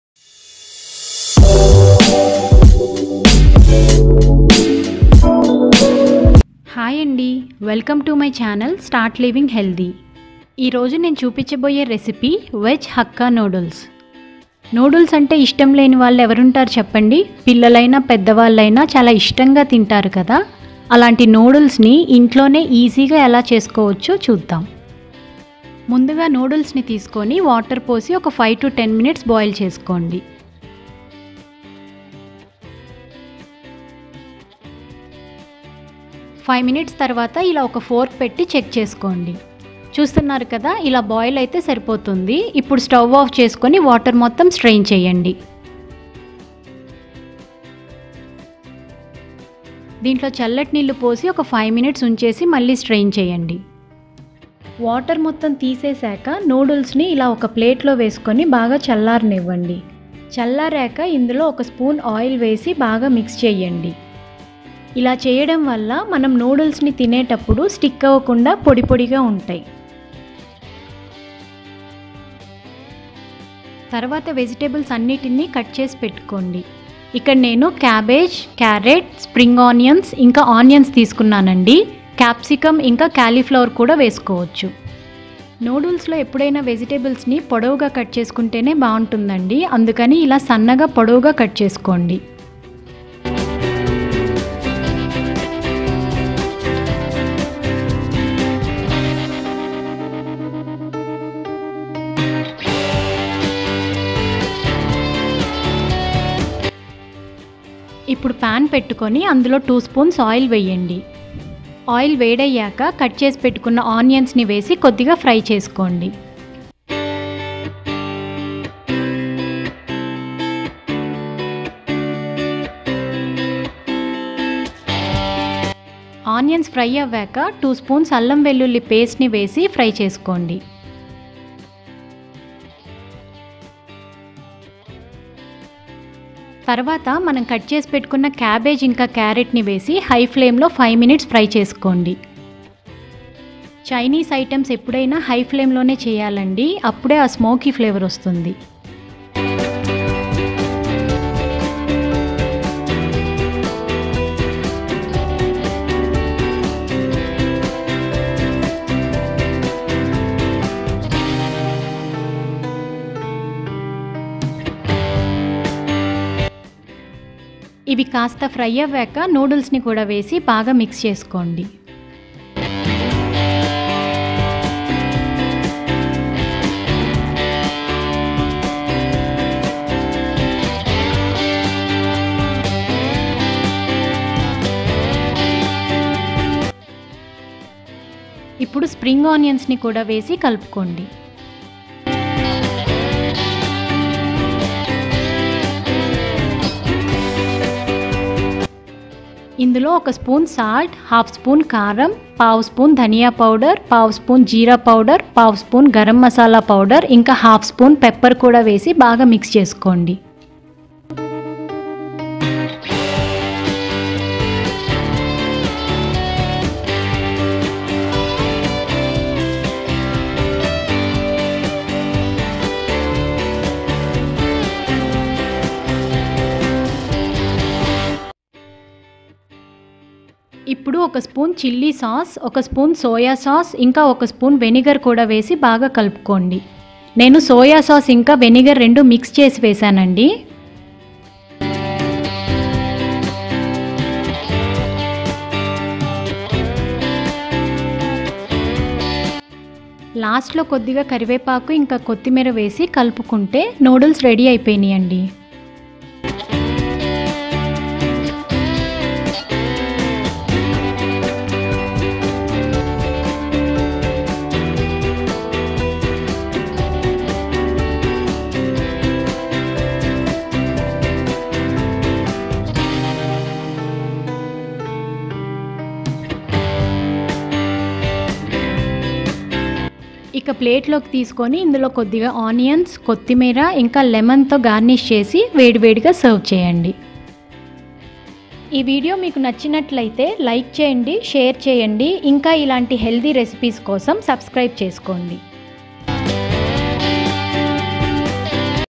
Veg Hakka Noodles in telugu || chinese recipe || street food || start living healthy
How to prepare veg noodles in telugu that to in street style